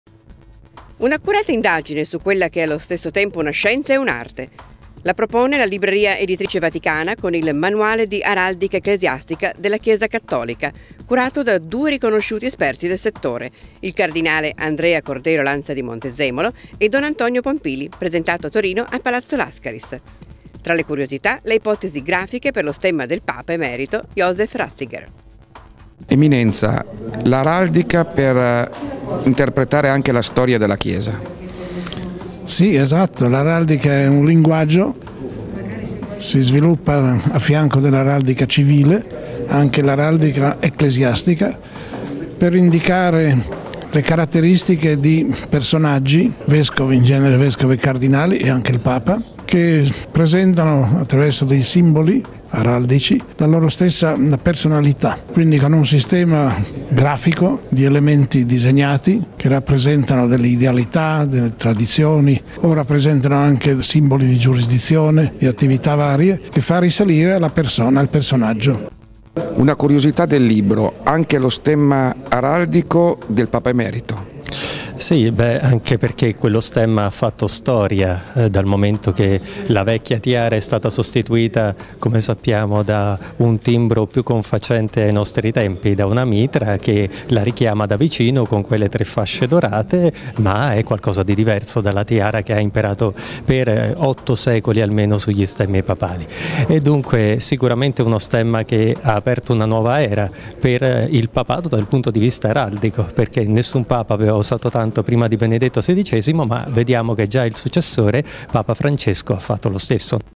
Manuale di Araldica Ecclesiastica
Un’accurata indagine su quella che è allo stesso tempo una scienza e un’arte. La propone la Libreria Editrice Vaticana con il “Manuale di araldica ecclesiastica nella Chiesa cattolica”, curato da due riconosciuti esperti del settore, il cardinale Andrea Cordero Lanza di Montezemolo e don Antonio Pompili, presentato a Torino a Palazzo Lascaris. Tra le curiosità, le ipotesi grafiche per lo stemma del Papa emerito Joseph Ratzinger
[intervista al Cardinale Andrea Cordero Lanza di Montezemolo, don Antonio Pompili ]